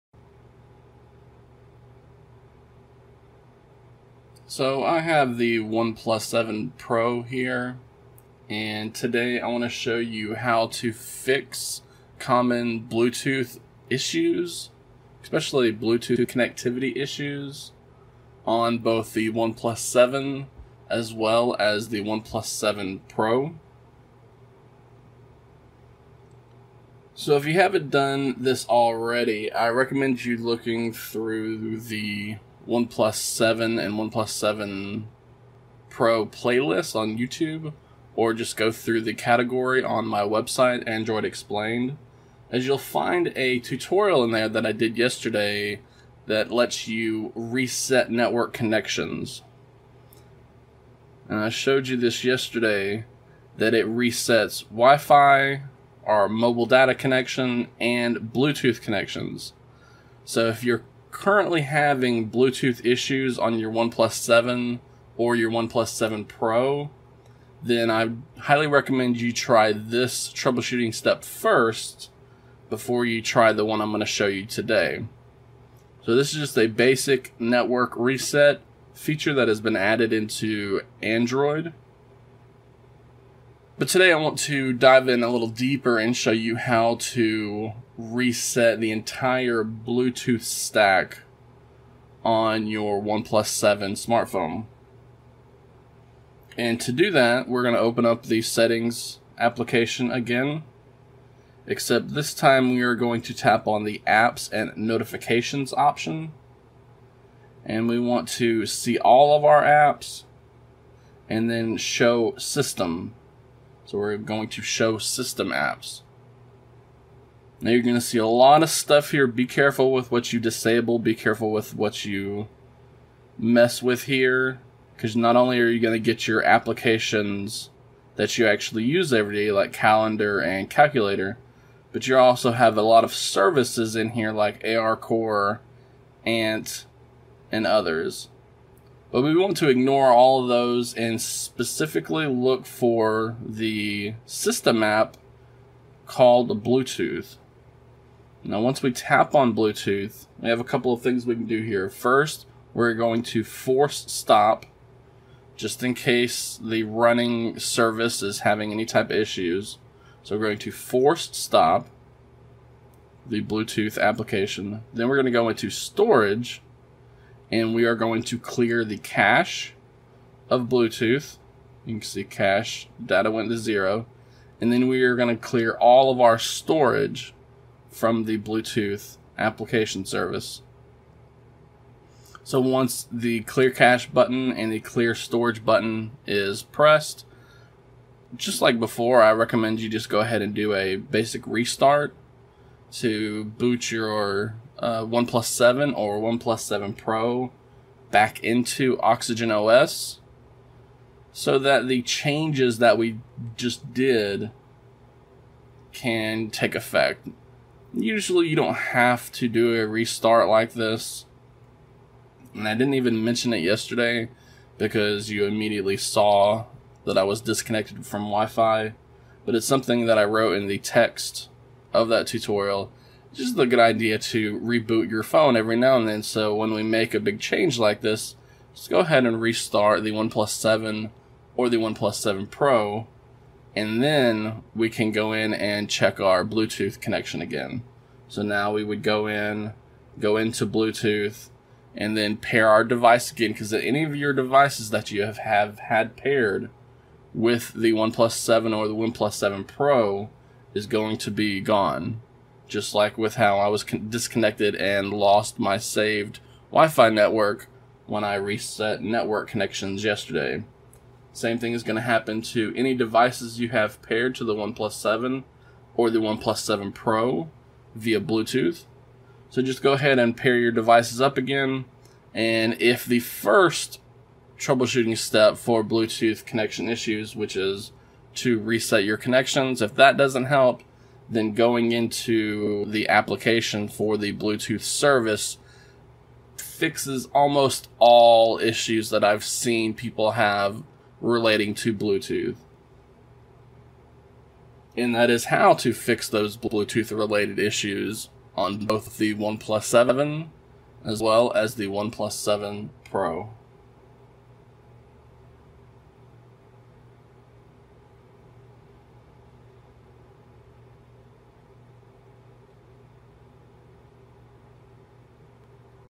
OnePlus 7 & 7 Pro | Fixing Bluetooth Connection Issues
Whether you're trying to pair a Bluetooth speaker to the smartphone or get it connected to your automobile. The process of making this connection has been made user-friendly while still having a layer of security to it. Still, most people hate going through the motions of pairing their smartphone to various Bluetooth devices.

And this is becoming increasingly more common as Bluetooth continues to gain popularity.

Bluetooth has become a common technology that many of us use every single day of our lives now. It's become so common that any type of Bluetooth related issues can completely ruin a smartphone. These Bluetooth connectivity issues are quite common, but I can show you how to fix them on both the OnePlus 7 and the OnePlus 7 Pro.